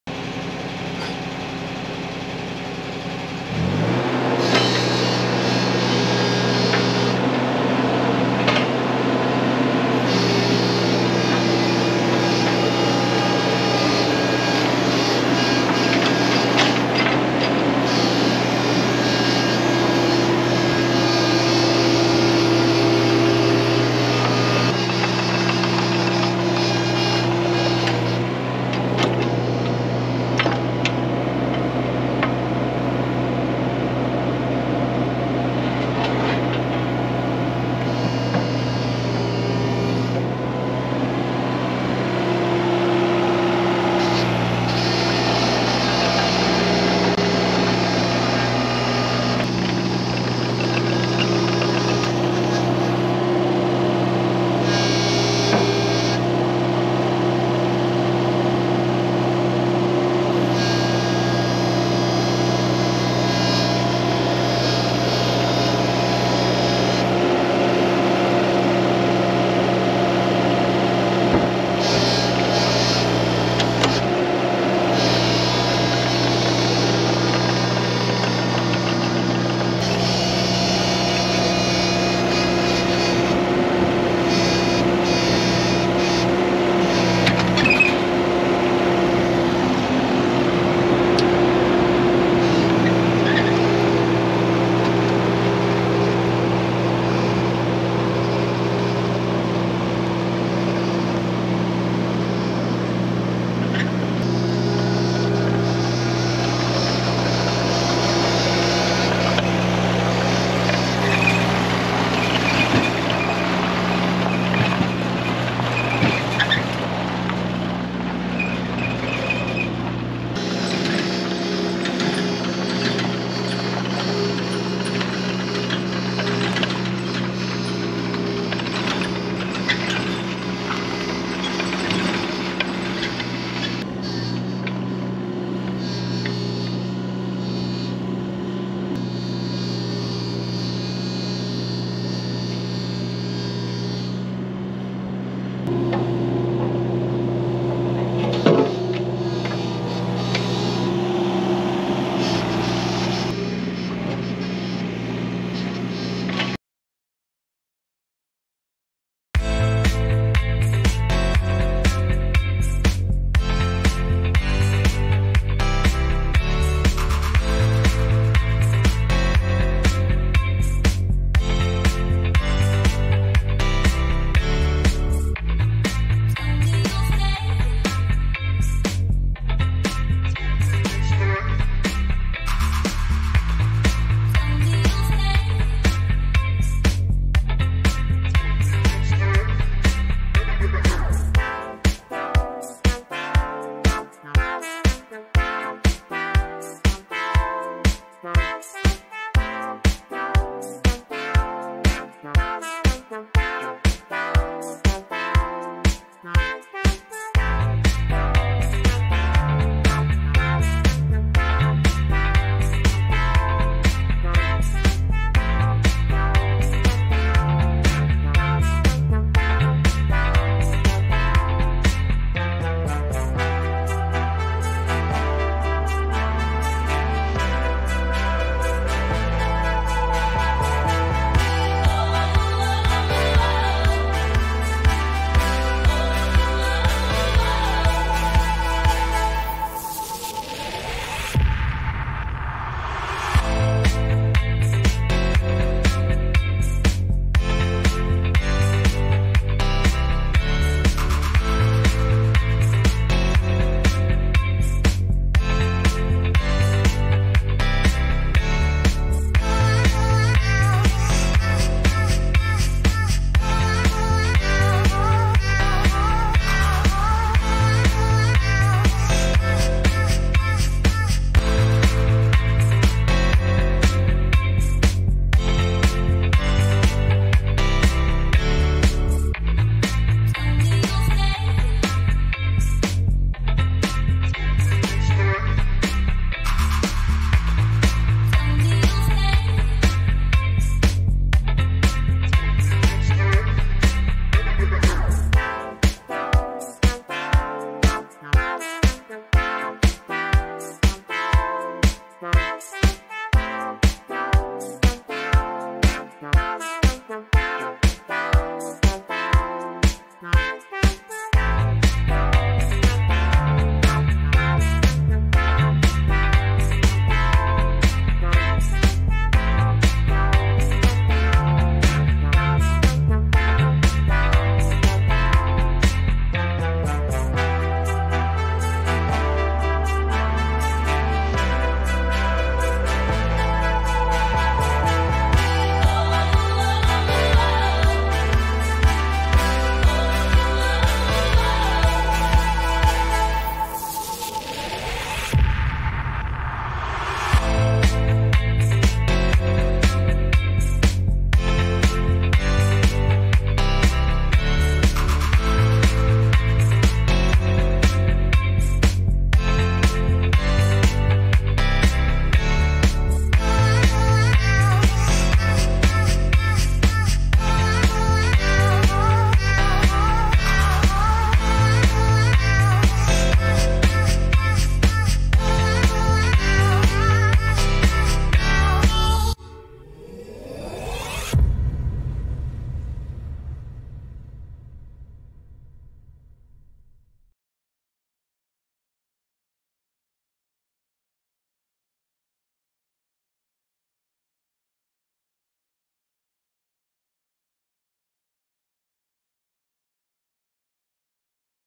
Self Build house uk foundation installing 9 meter piles (PART 1)
Building my own house part 1 - starting the foundations.